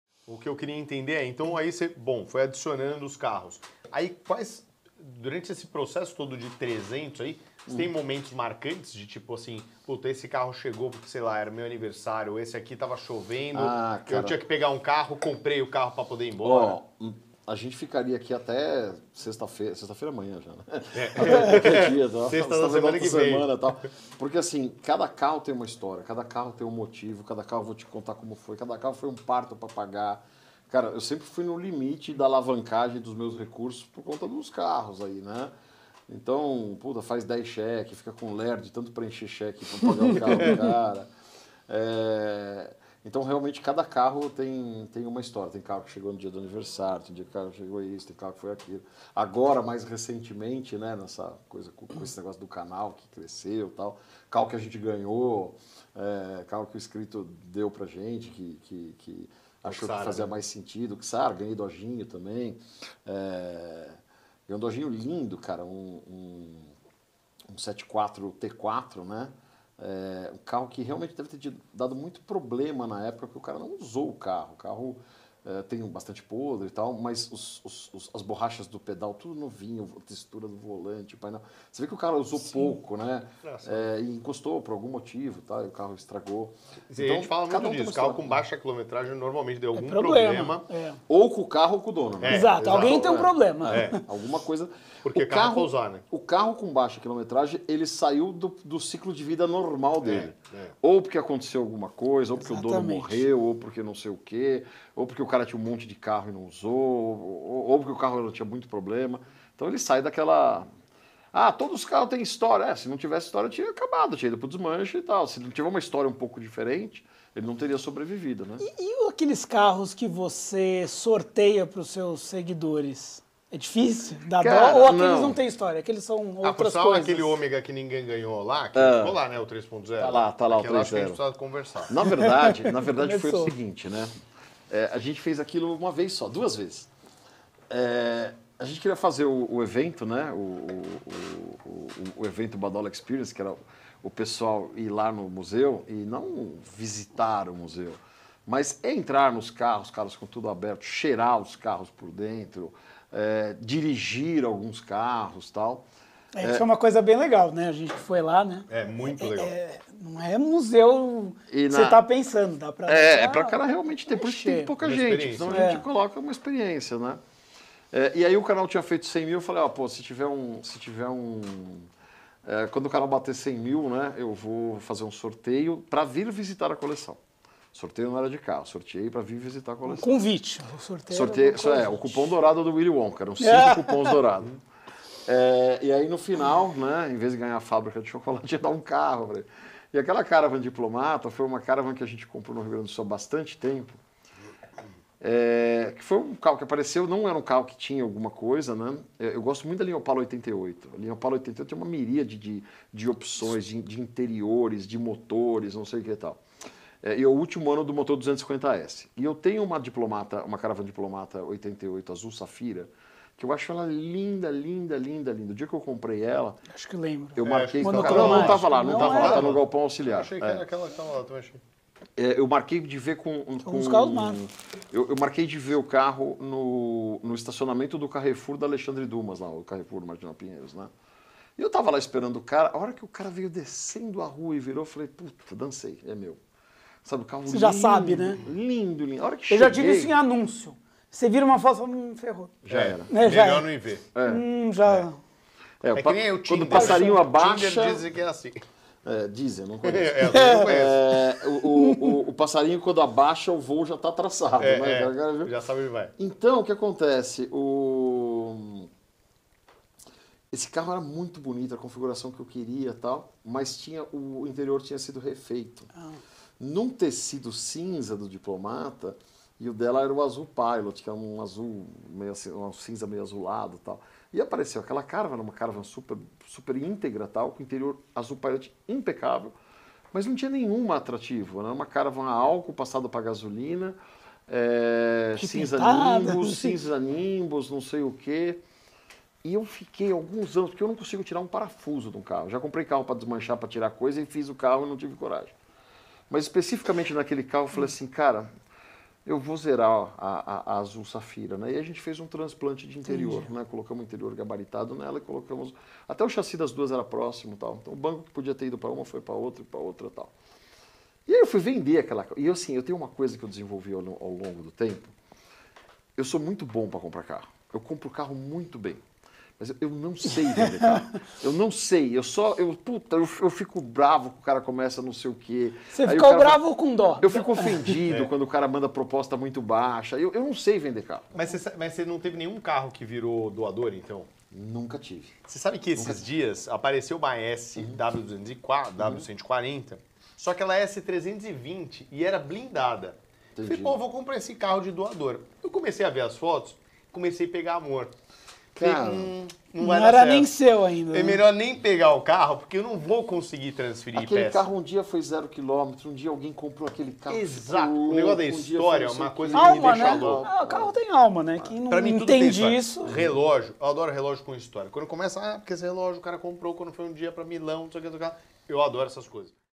ÓTIMO PRA COMPRAR CARROS E PÉSSIMO PRA VENDER - ALEXANDRE BADOLATO | CortesApC
e começa a saga dos cortes da live do badolato.